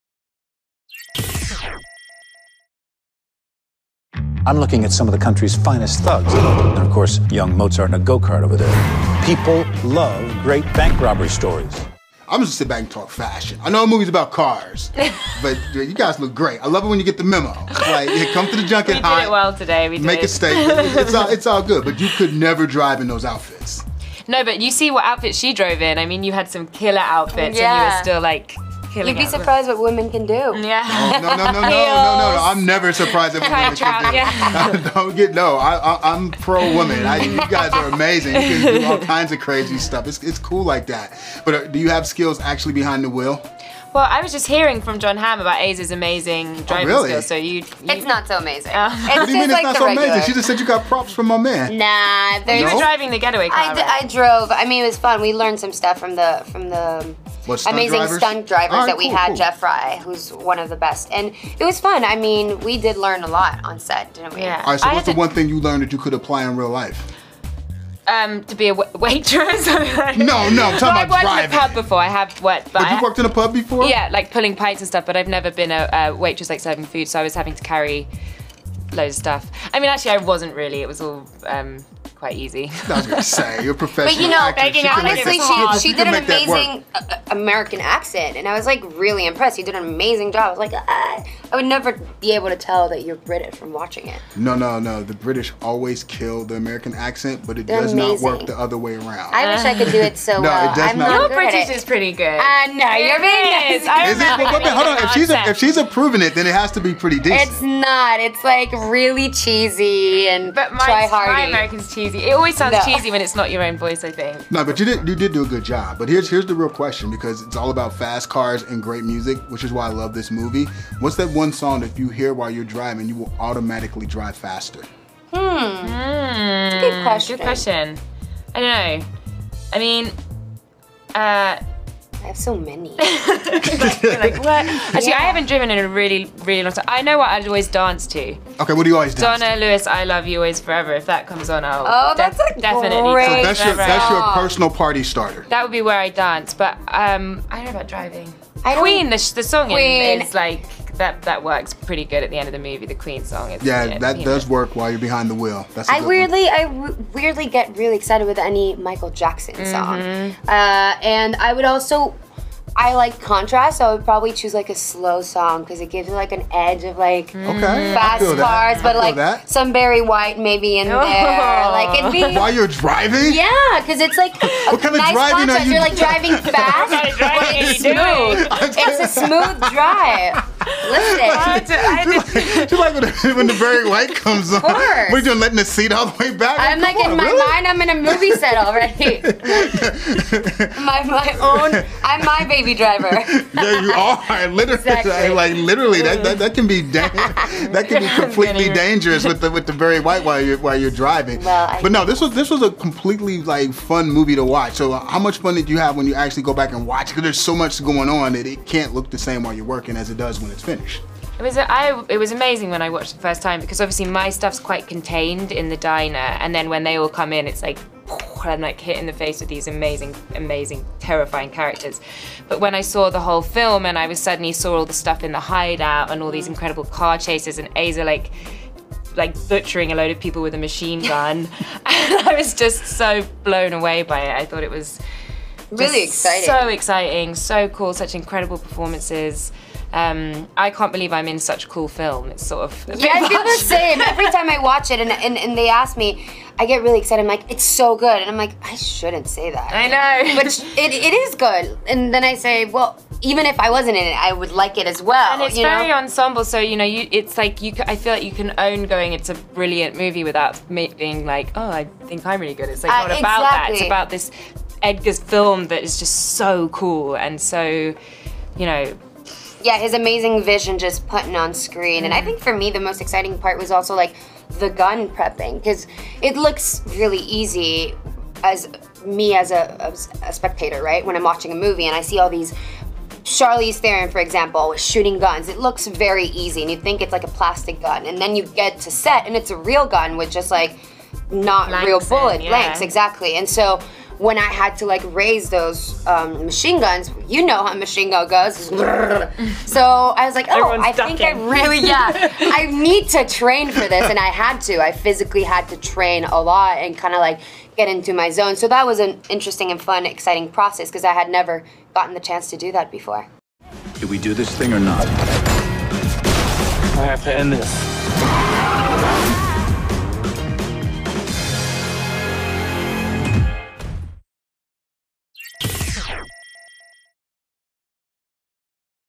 Eiza González and Lily James talk "Baby Driver"
After being coerced into working for a crime boss, a young getaway driver finds himself taking part in a heist doomed to fail.